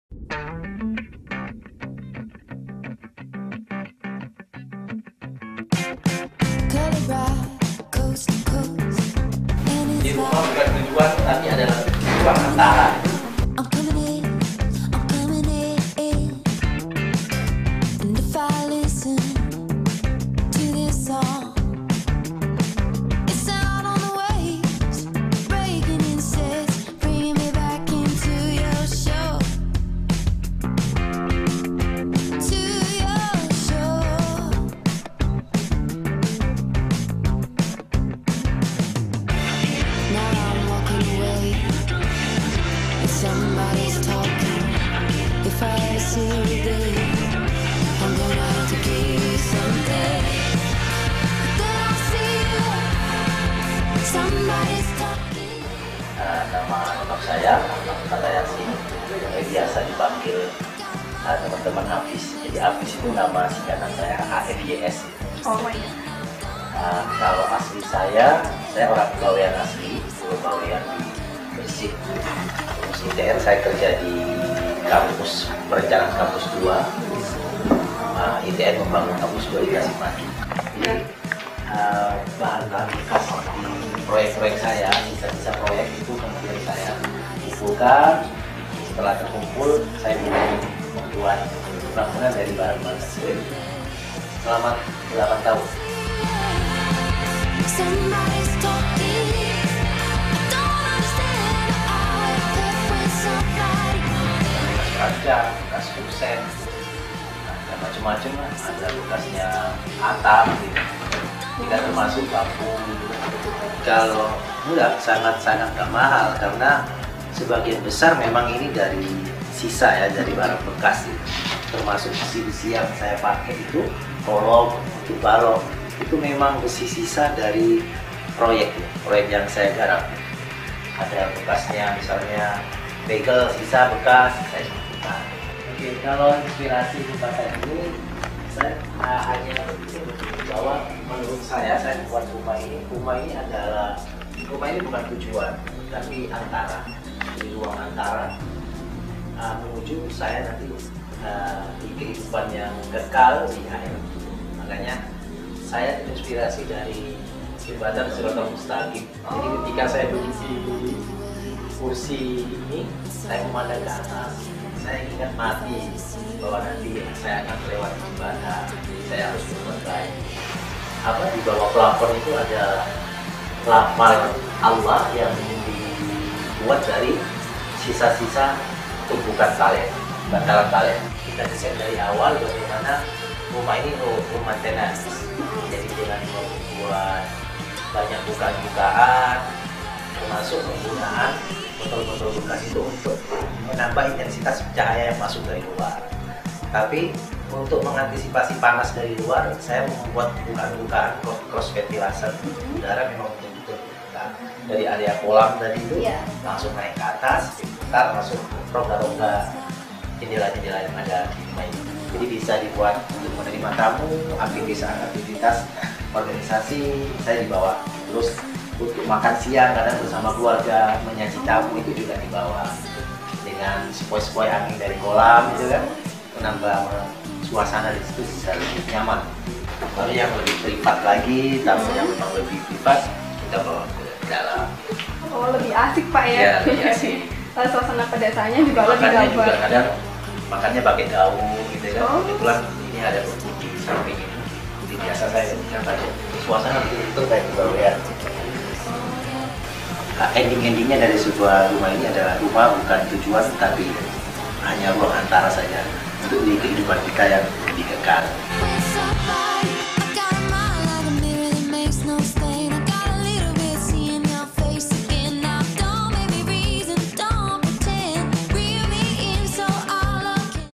Afys Recycle House (eps. 1)
Sebuah rumah yg dibangun dari material daur ulang sisa sampah proyek dan rumah tangga
Berlokasi di Perum Nadia Residence A17 Jl. Ir. Sukarno Ddaptulis Utara Junrejo Kota Wisata Batu
Owner_Akhmad Fatah YaSin (AFYS)
Asli Orang Bawean Jawa Timur